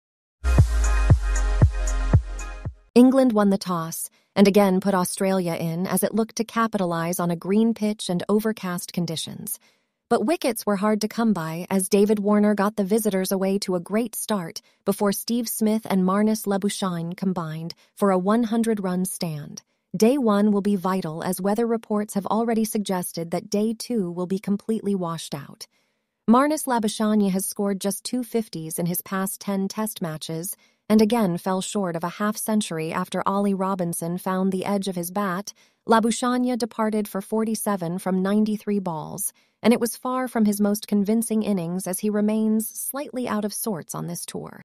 Aussie commentator Mel Jones ruins England villain Ollie Robinson at Lord’s
England won the toss and again put Australia in as it looked to capitalise on a green pitch and overcast conditions. But wickets were hard to come by as David Warner got the visitors away to a great start before Steve Smith and Marnus Labuschagne combined for a 100-run stand.Day one will be vital as weather reports have already suggested that day two will be completely washed out.Marnus Labuschagne has scored just two fifties in his past 10 Test matches and again fell short of a half century after Ollie Robinson found the edge of his bat.Labuschagne departed for 47 from 93 balls and it was far from his most convincing innings as he remains slightly out of sorts on this tour.